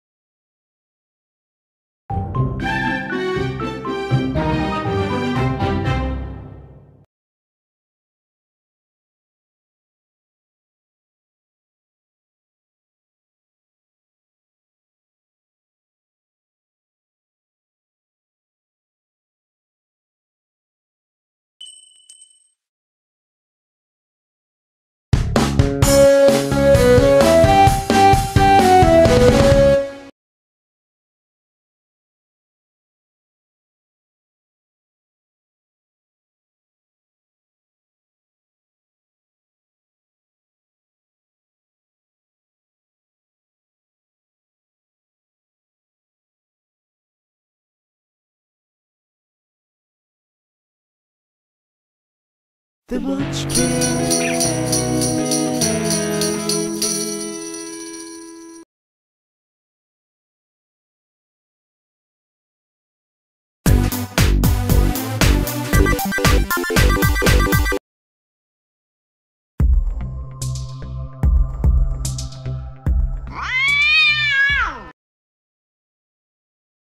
Youtube Intros Show Reel
Updated as of June 2015. Demo reel of the intros I've made for various Youtube Channels.